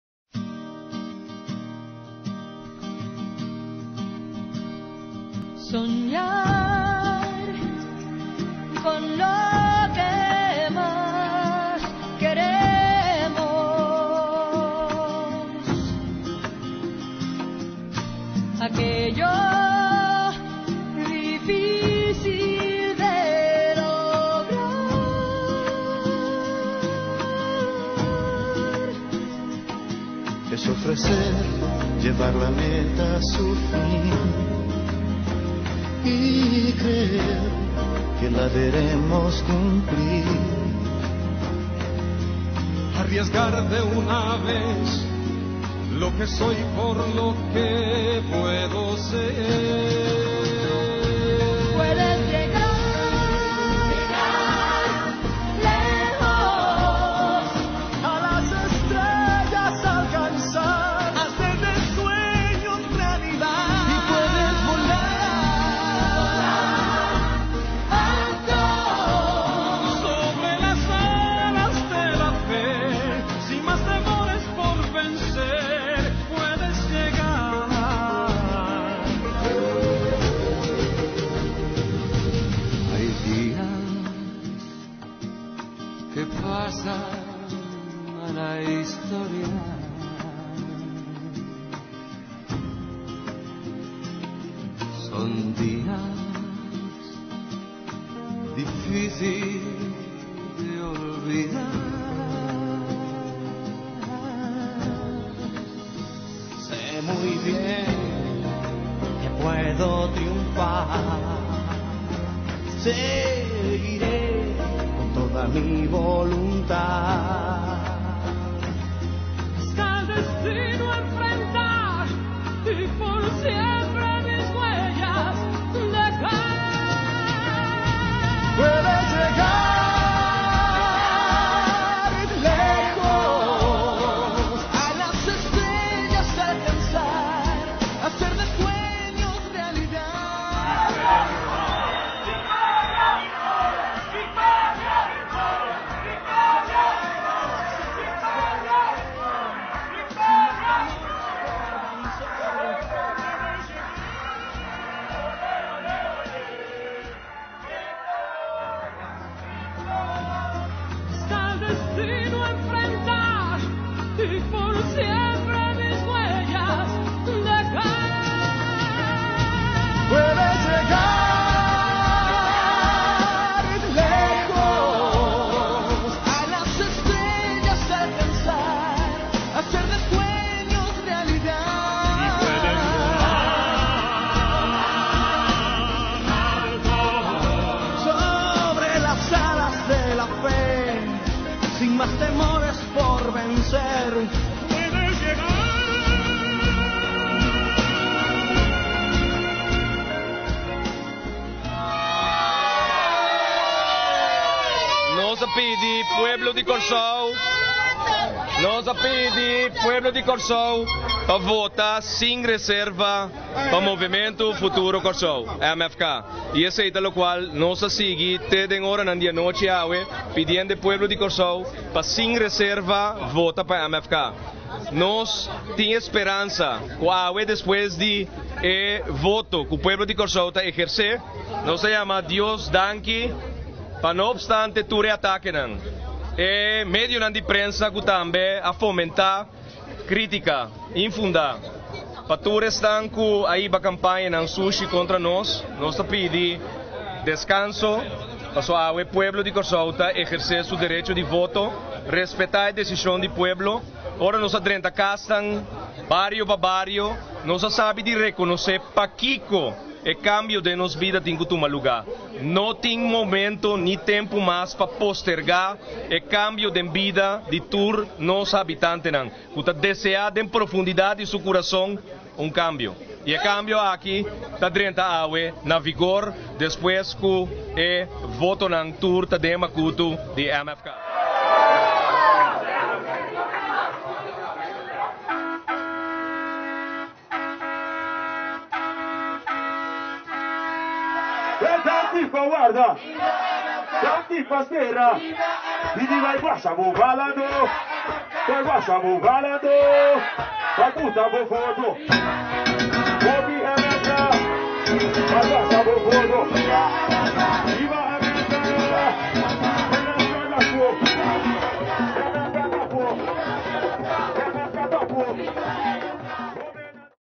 Momentunan kaminda mi a asistí urna elektoral
Mi no tin palabra pa deskribí e emoshon ku m'a sinti ora mi a bai vota pa e partido ku ta bai kambia e futuro di Kòrsou. Mi ta kompartí ku boso e momentunan kaminda mi a asistí urna elektoral huntu ku mi famia, amigunan, kandidatonan i simpatisantenan di MFK.

Un kambio ku ta bon pa nos famianan falta djis algun ora pa yega. Kòrsou a disidí i nos sa ku nos ta bai gana. Nos triunfo ta triunfo di hinter un pueblo!